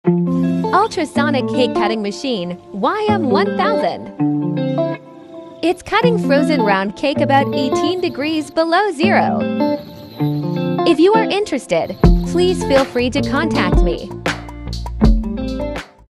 ZGYMZN Ultrasonic cake cutter for bakery industry
Application
1.Bakery and baked goods categories like bars breads (bagels, buns, rolls, biscuits and loaf breads)
2.cookies, desserts (cakes, cheesecakes and pies) muffins
3.pizza,
4.snack cakes
5.sweet goods (doughnuts, Danishsweet rolls, cinnamon rolls and coffee cake）
6.tortillas